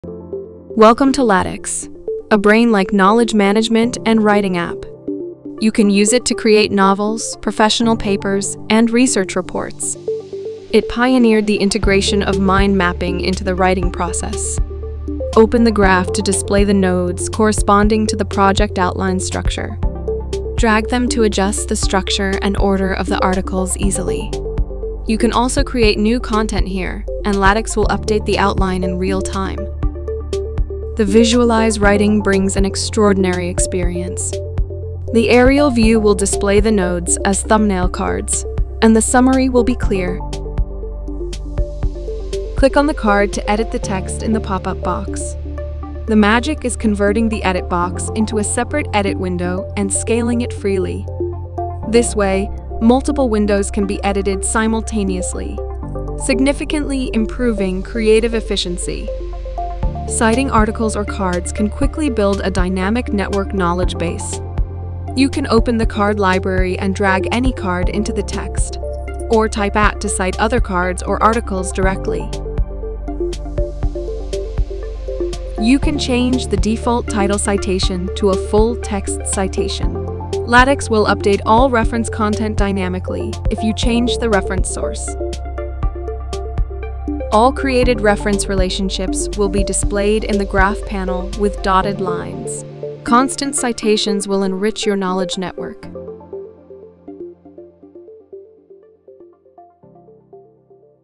Lattics - "Brain-like" Knowledge Management and Writing App
From data structure, rich text editing, to the entire software interface, Lattics adopts a whole new "brain-like" concept.
We believe that "brain-like" KM tools should have 6 characteristics, which will be reflected in Lattics:
- Minimize Module Storage
- Hierarchical Content Dynamic Relationship Network
- Multidimensional Linear Narrative
- Divergent Information Expansion
- Minimize Switching Work Context
- Information Visualization

Features overview:
- Content GRAPH: look and manage the relation between contents by projects and tags, create mind maps, plot plans
- OFFLINE FIRST:    No need to register/login to use, the data is stored on the local by default
- WHOLE DATA BACKUP:   All data can be backed up manually or regularly
- File MANAGEMENT: PDF, Doc, Excel, PowerPoint, Keynotes, Numbers, Pages, JPG, PNG, Video etc
- Zettelkasten METHOD:   The card library can record various inspiration or fragment info
- CONTENT INTERCONNECTION:   Using the syntax "@" (cite), "[^" (footnote), easy to establish a link between articles and cards, modify one place, update any places
- EXTENSION INFO:   Tag articles and cards, look for bidirectional links, document structure, etc.
- BIDIRECTIONAL LINK: use @ to link other articles, and in the extension info, you can look up the forward links, backlinks, and unlinks of the article
- DUAL SCREEN COMPARISON:   You can open two editors at the same time to compare and edit two articles respectively
- Hierarchical OUTLINE:   The project outline has unlimited levels, and the article can be dragged to adjust the level position
- Hierarchical TAG
- ARTICLE INTEGRATION:   Articles can be merged, split, and read sequentially
- BLOCK ELEMENTS:   Use the syntax syntax "/" to insert paragraphs, pictures, codes, tables, math equations, and drag&drop to move their position
- COLUMNIZE:   Text and pictures can be typeset in multiple columns, and drag and drop to layout in columns
- MATH EQUATION:   Offers common operators, including Greek characters, Geometry, Sets, Functions, Calculus, etc., which is far more usable than MathType
- MARKDOWN:   Automatically parse the common syntax of MD, and render it into a WYSIWYG
- CUSTOM STYLE:   Design the styles of Fonts, Alignment, Indentation, Spacing, List, Colors, Borders, etc. and save as style templates
- HIGH QUALITY EXPORT:   Cover, Table of Content, Page numbers, Headers and Footers, A4/A5 PDF, PNG, Docx, MD, TXT;
- Search Highlight
- Count
- Night Theme
- Recycle Bin